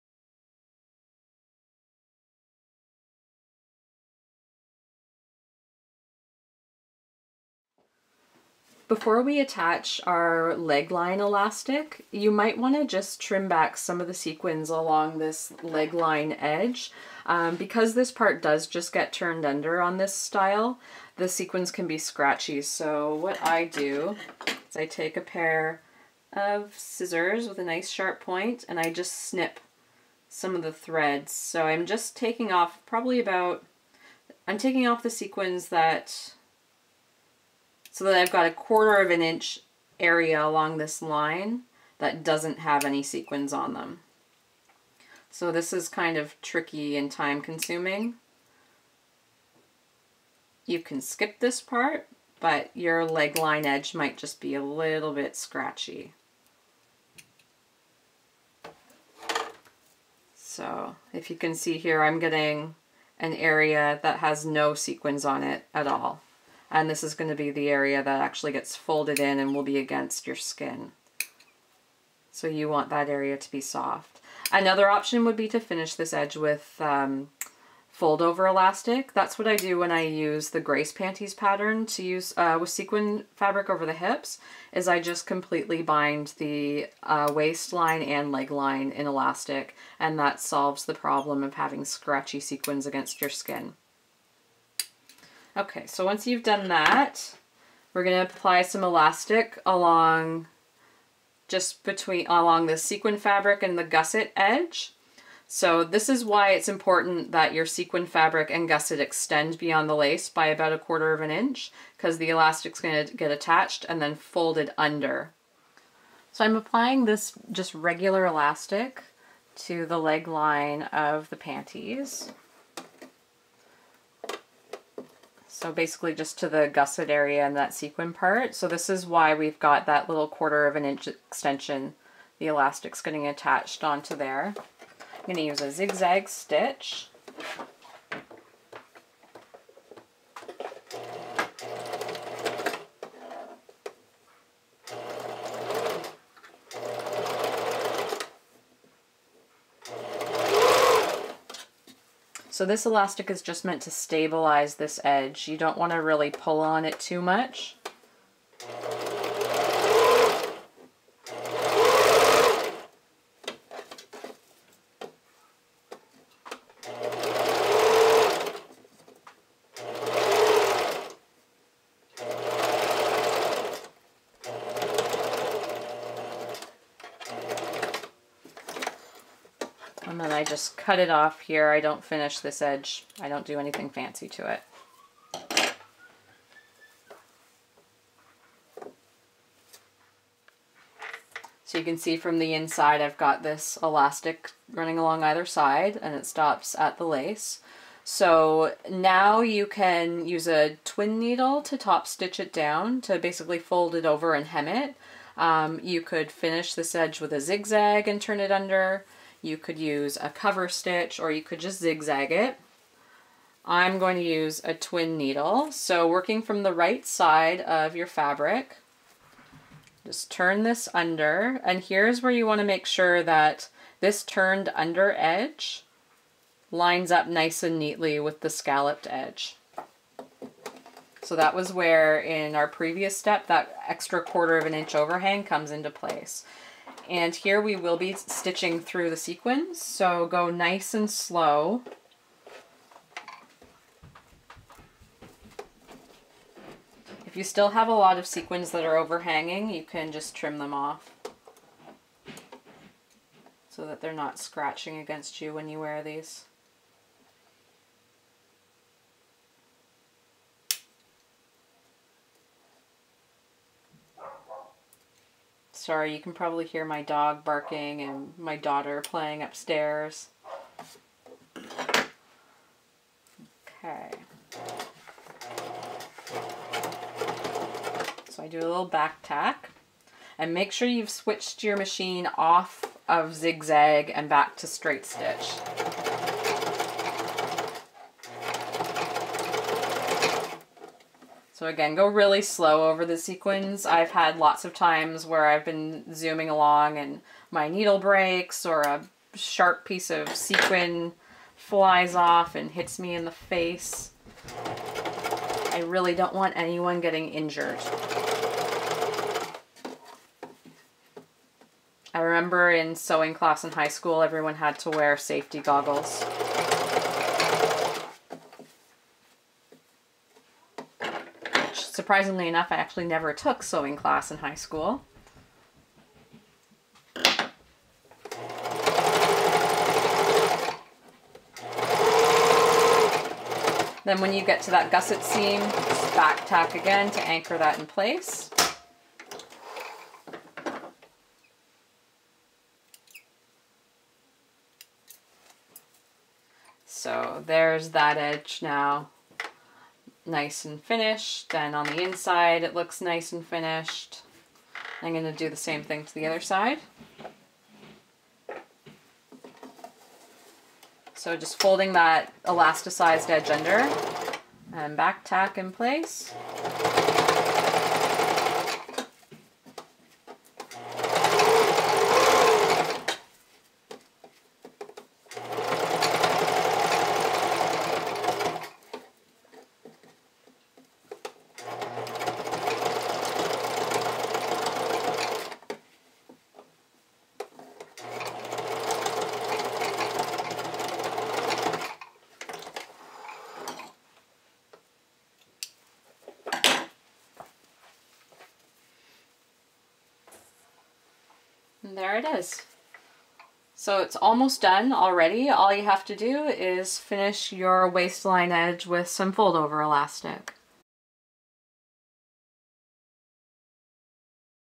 Stabilizing the Legline with Elastic on the Claudia Panties
I'll walk you through how to add some elastic to stabilize the leg-line of the Claudia Panties, with some specific tips for workign with sequin fabrics.